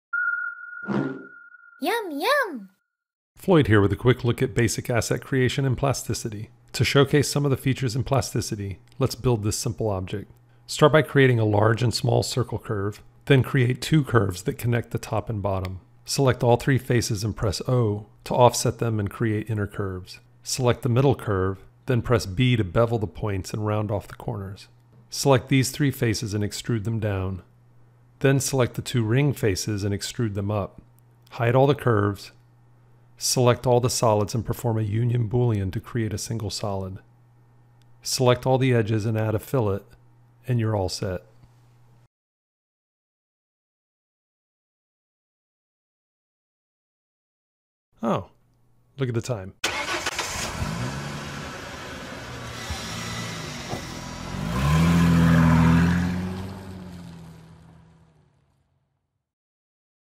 Plasticity | Modeling Exercise 06
This video takes a quick look at basic asset creation in Plasticity.